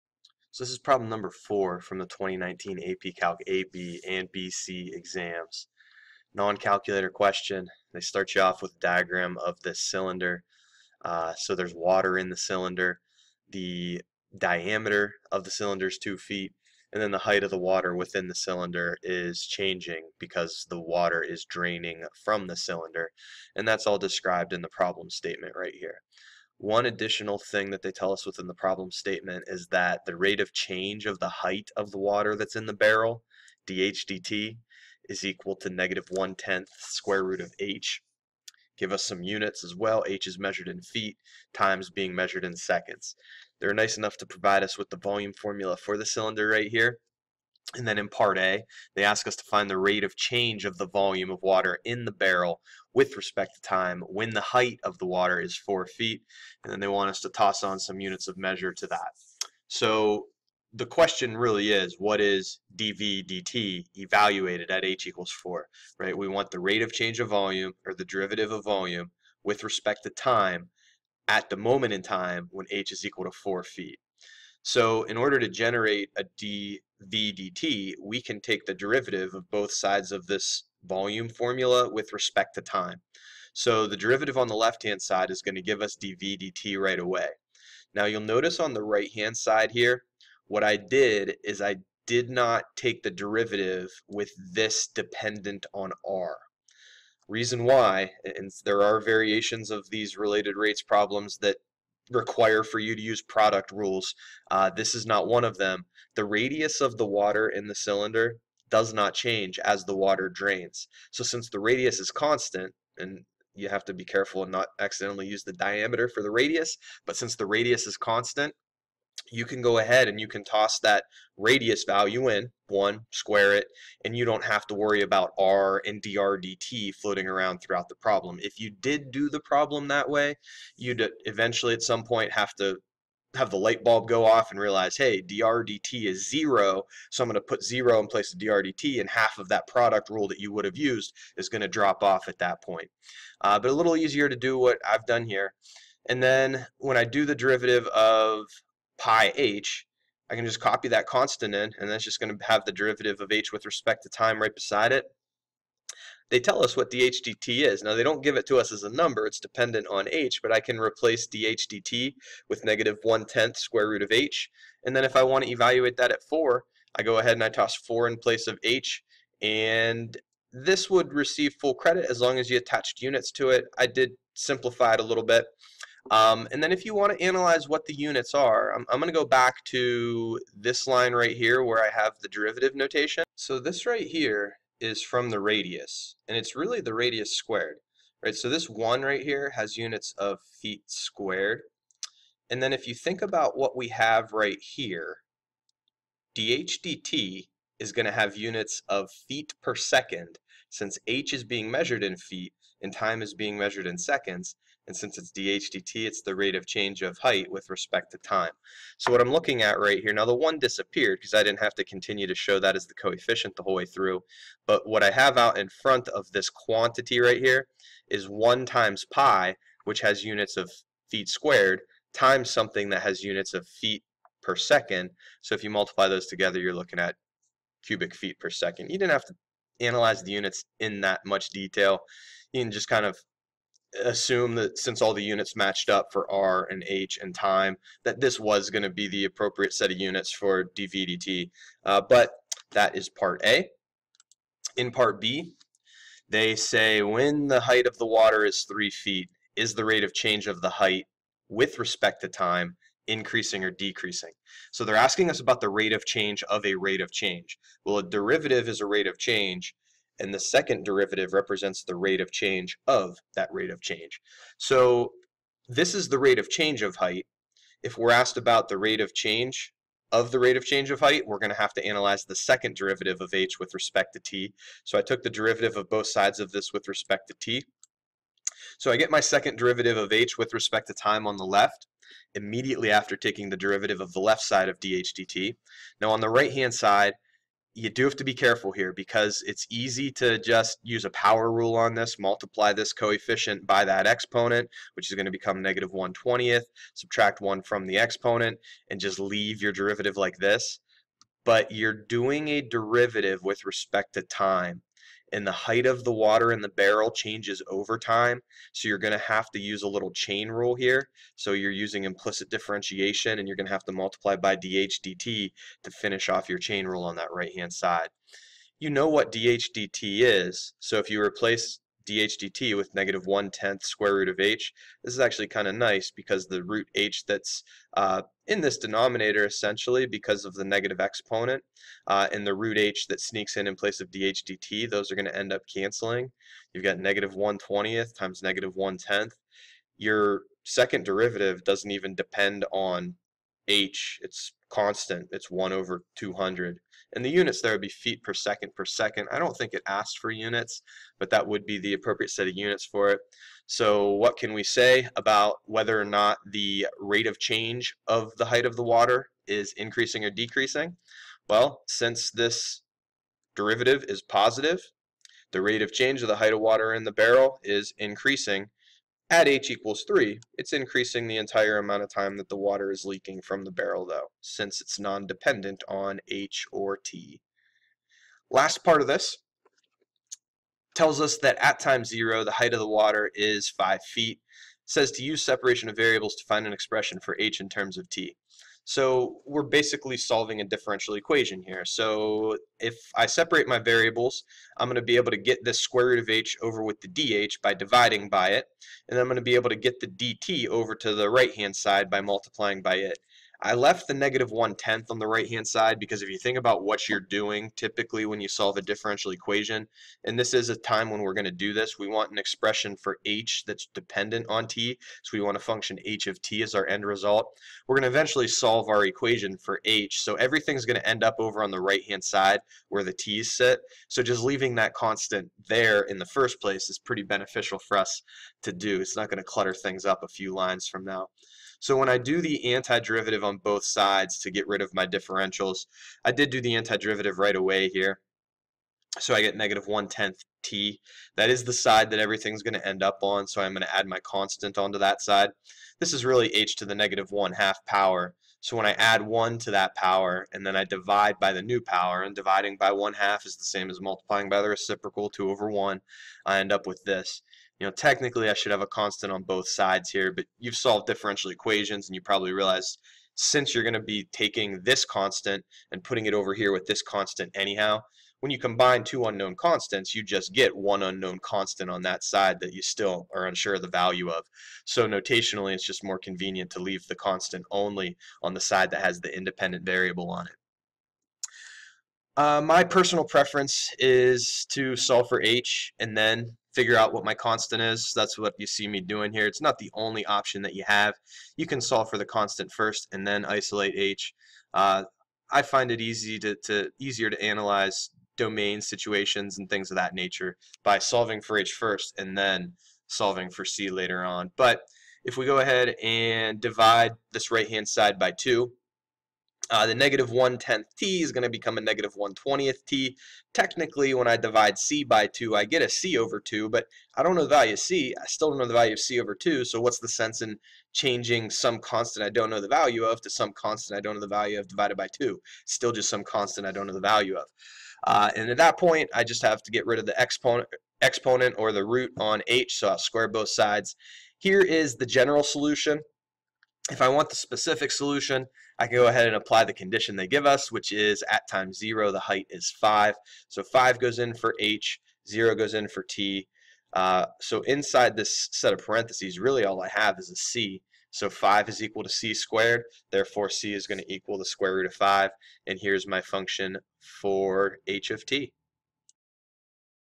2019 AP Calculus AB & BC Free Response #4
A cylindrical barrel with a diameter of 2 feet contains collected rainwater